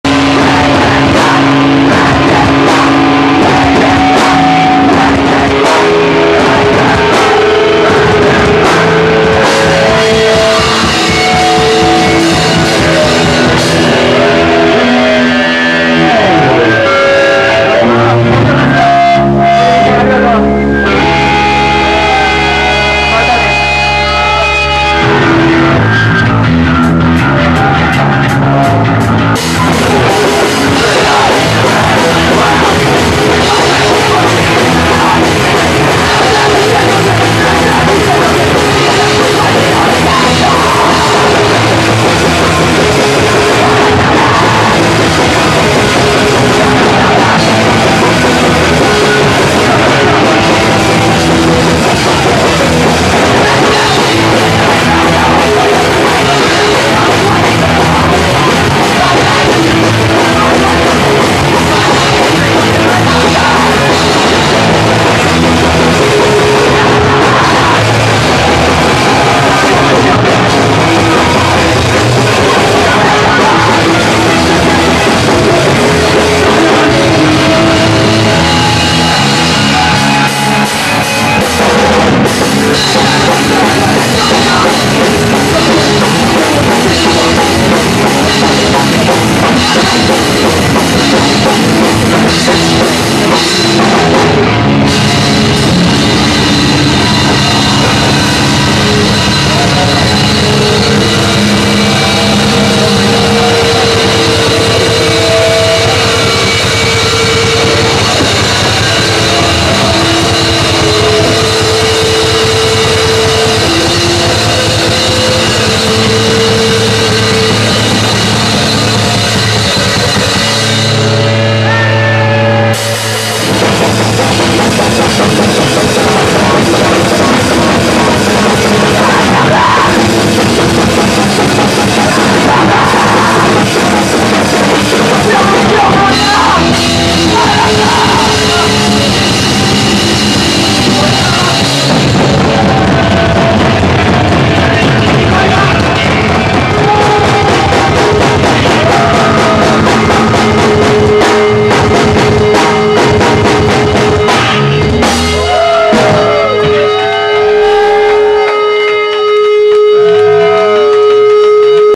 everybody's enemy live- koty's last show
Last show for EE's guitarist Koty after 6 years in the band...